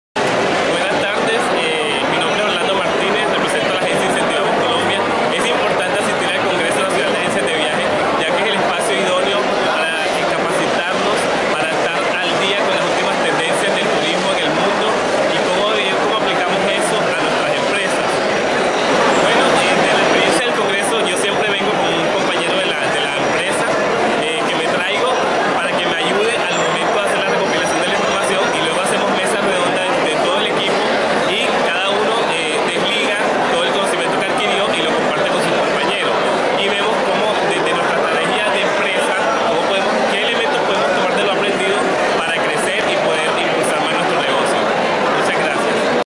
Orlando Martínez Incentivamos Colombia
El Congreso Nacional de Agencias de Viajes es uno de los más importantes eventos académicos del sector turismo que reúne a Agentes de Viajes y Turismo, Mayoristas y Operadores de Colombia, afiliados o no a la Asociación.